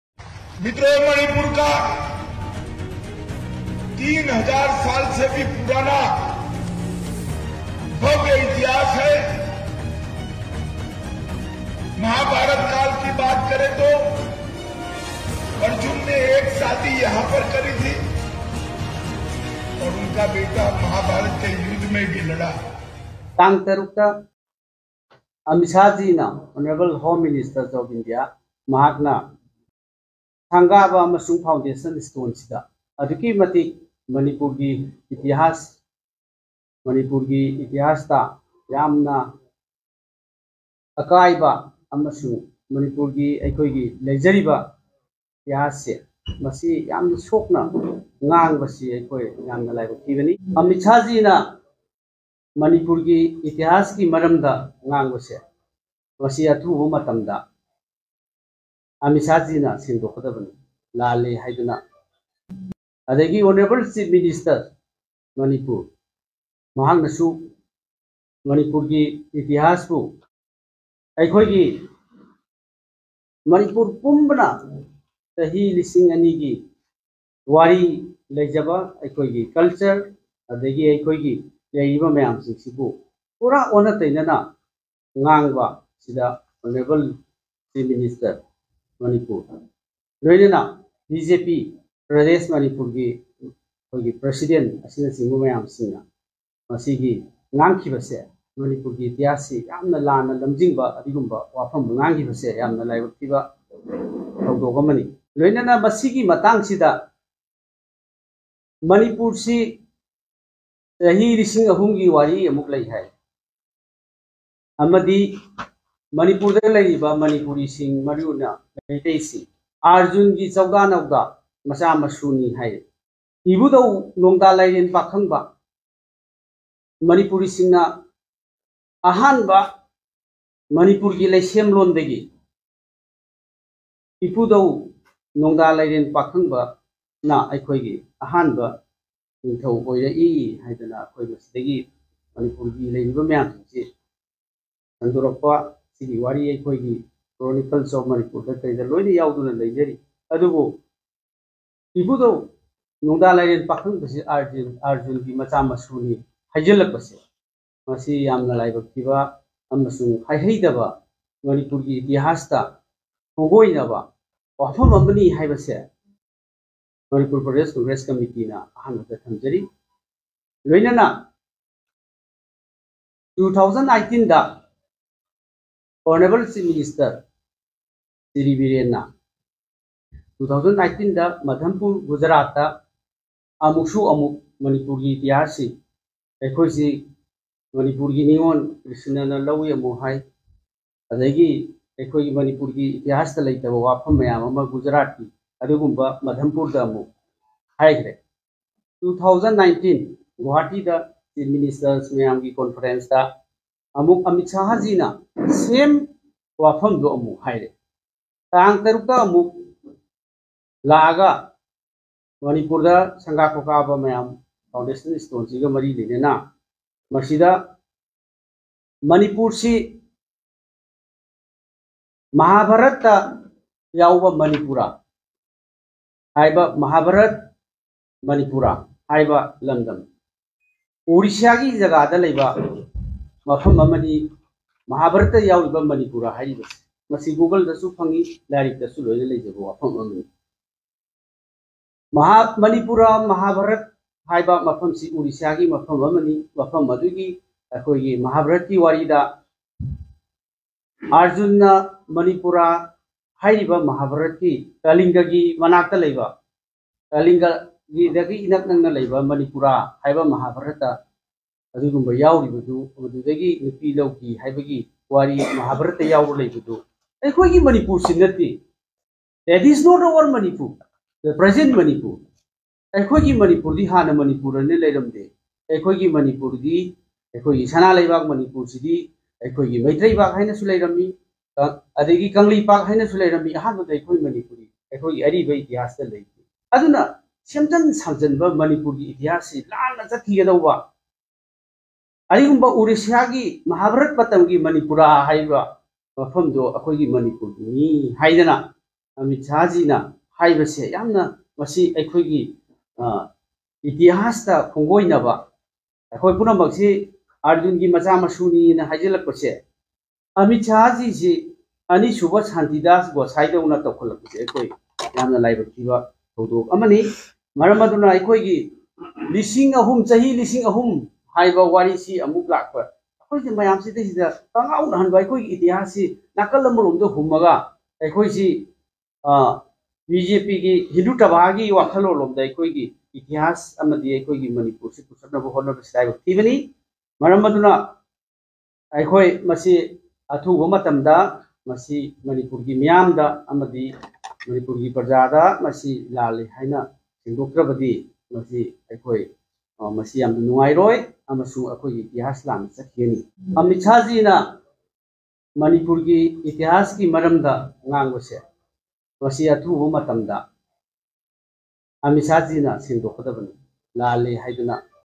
Home Minister Shri Amit Shah, should apologise to the people of Manipur.
2nd Shantidas Gosai of Manipur, Union Home Minister Shri Amit Shah, should apologise to the people of Manipur for distorting the rich history of Manipur:

K. Meghachandra, Manipur PCC President.

Follow Indian National Congress-Manipur!